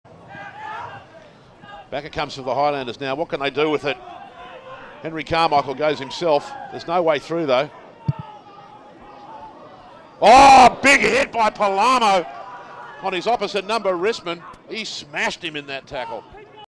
Gareth Palamo smashes Andrew Turner with tackle
Gareth Palamo No.8 (Eastwood) Smashes Andrew Turner No.6 (Gordon) in a tackle.
Eastwood Vs Gordon Round 16 Shute Shield 2013

Gareth Palamo:
Weight:103kg
Height: 1.84m

Andrew Turner
Weight: 100kg
Height: 1.94m